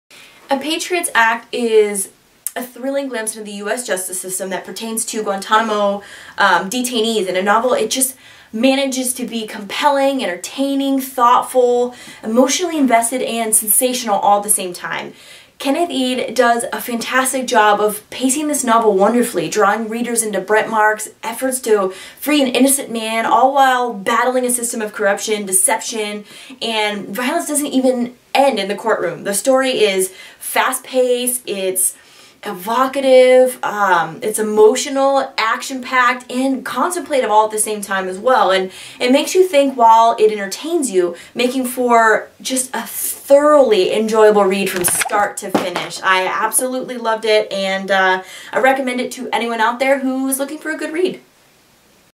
Video book review of Legal Thriller: "A Patriot's Act"
"A Patriot's Act" is a thrilling glimpse into the US justice system that pertains to Guantanamo detainees in a novel that manages to be compelling, entertaining, thoughtful, emotionally invested and sensational all in one. Kenneth Eade does a fantastic job of pacing this novel wonderfully, drawing readers into Brent Marks' efforts to free an innocent man all the while battling a system of corruption, deception and violence that doesn't end in the court room. The story is fast-paced, evocative, emotional, action-packed and contemplative. It makes you think while it entertains you, making for a thoroughly enjoyable read from start to finish.